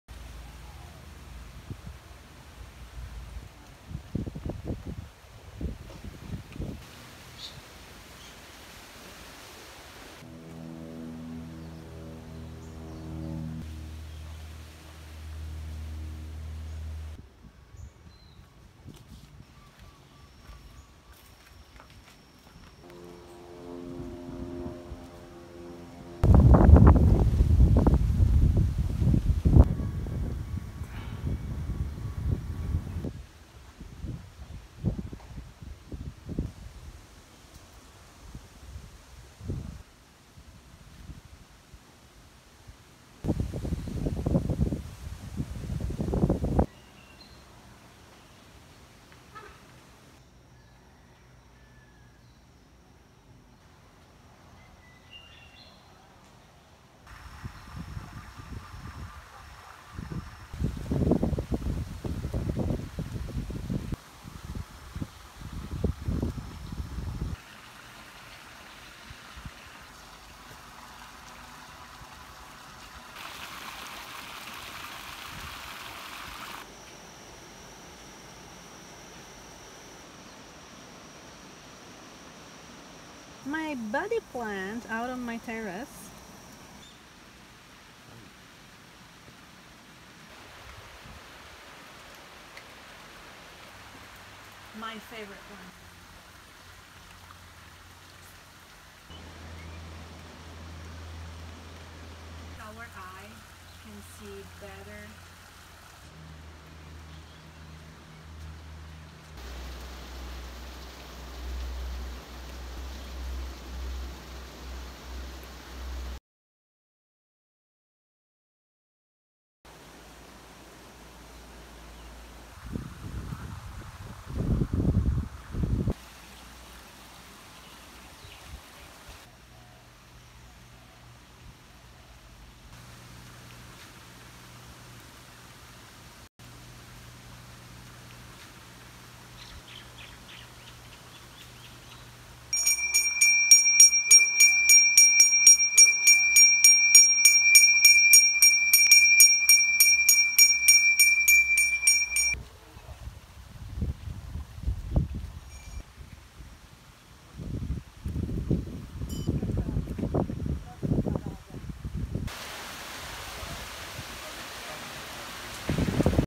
Kauai's Hindu Monastery & Temple
Have you ever wanted to go to a Hindu monastery and temple?

I hadn't, and to be frank, it wasn't at the top of my to do list for Hawaii. But now and then things come together. I recalled two years ago, Nicole Shaw telling me of a special place on Kauai of a temple and monastery situated on beautiful grounds, and that you could just sit, meditate and absorb it all. I had no idea that this place would be only a mile and a half from our Airbnb when I made our reservations for this trip, so when our host also spoke of all the things Nicole told me, I knew we had to visit and our new friend, Marina, the Italian chef, also experienced it with us too!